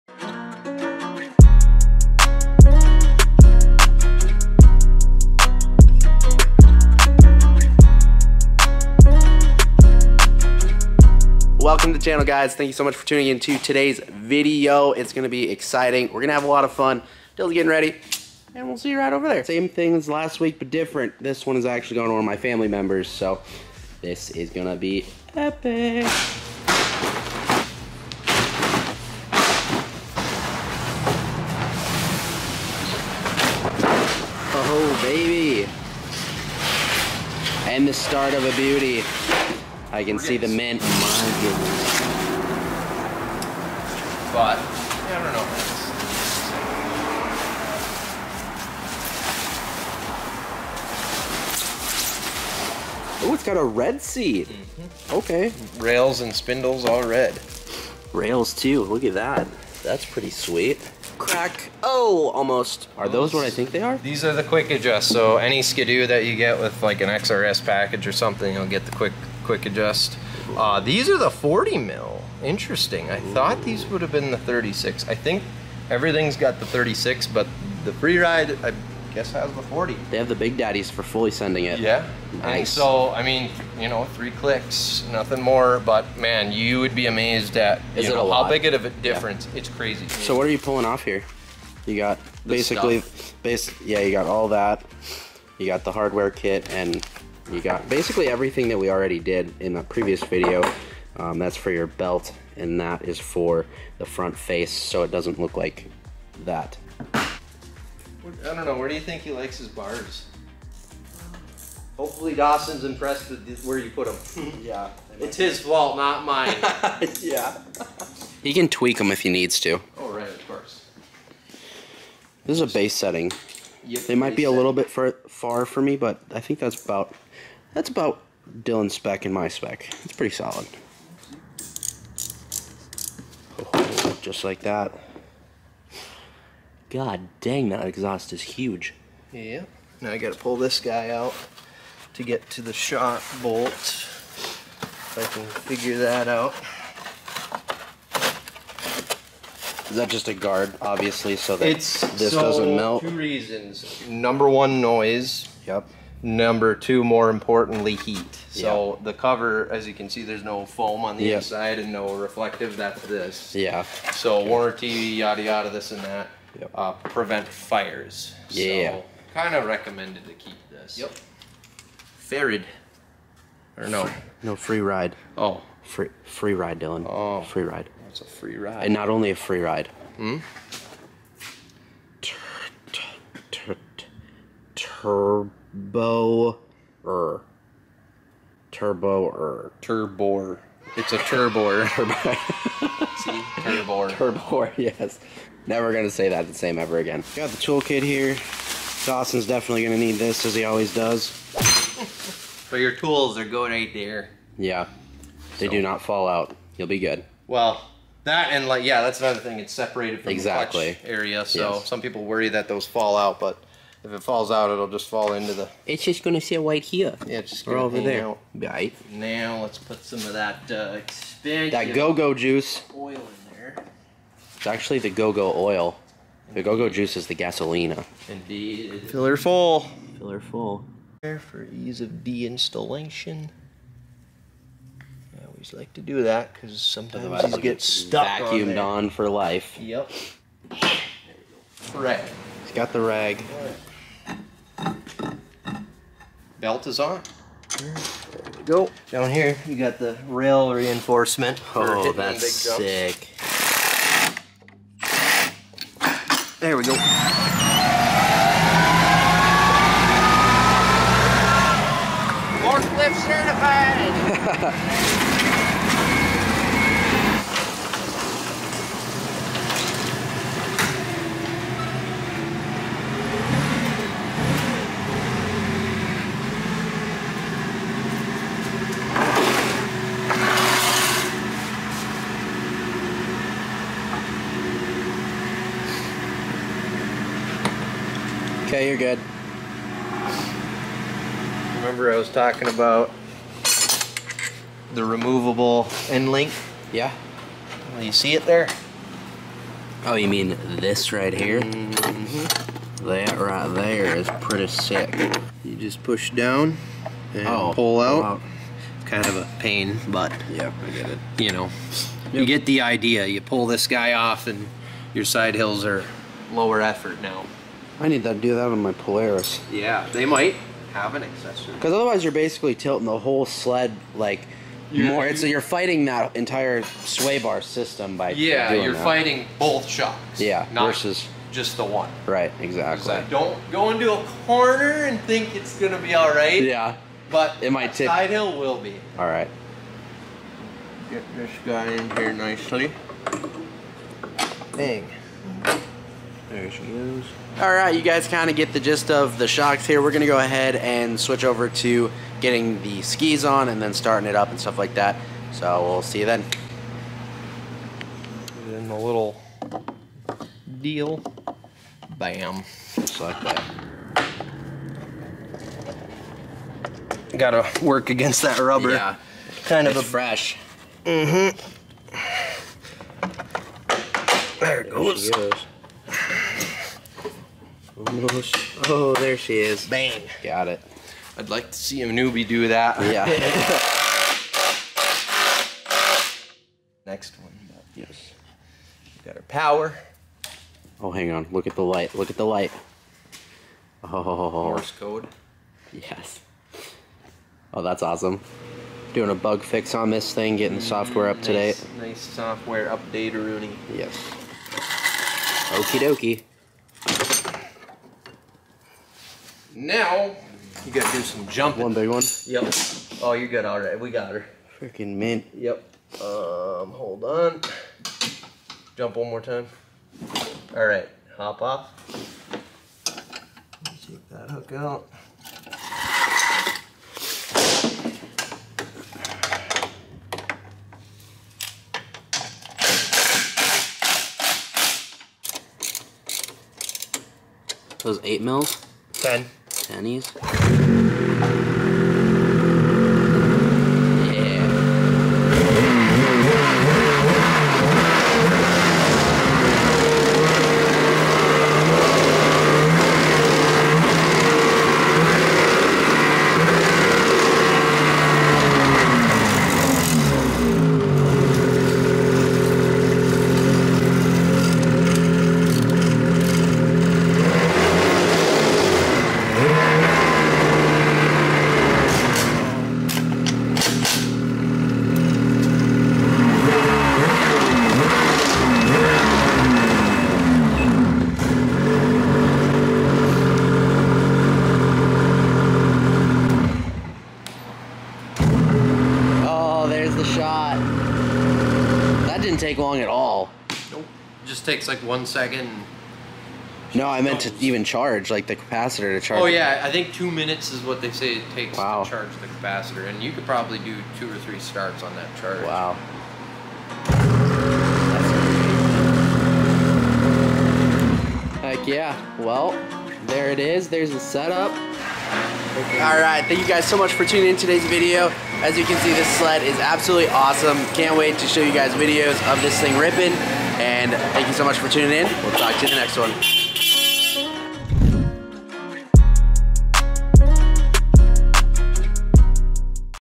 2024 Ski-Doo Freeride Turbo R | Unboxing
This 2024 Freeride is a beast, huge shoutout to Umotors for letting me film and to Dylan for setting up the beauty.
We got a BRAND NEW 4K CAMERA BABY! The quality is going to be top notch now!!

I hope you enjoyed the video and we can't wait to see this thing rip in the mountains.